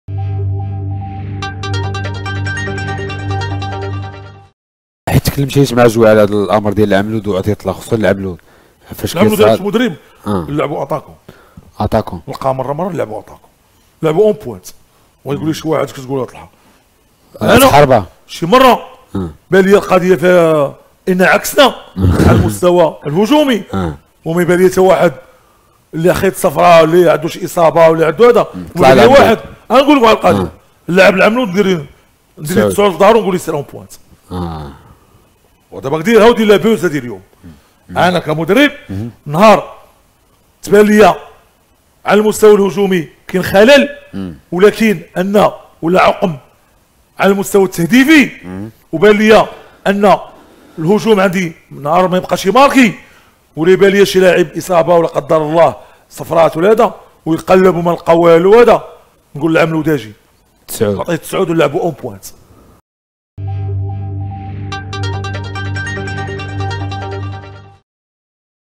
طلحة: عادل رمزي خصو يفكر ويوظف أيوب العملود كمهاجم
طالب محمد أرمومن اللاعب السابق ومحلل قناة شوف تيفي من مدرب الوداد عادل رمزي بتوظيف أيوب العملود كمهاجم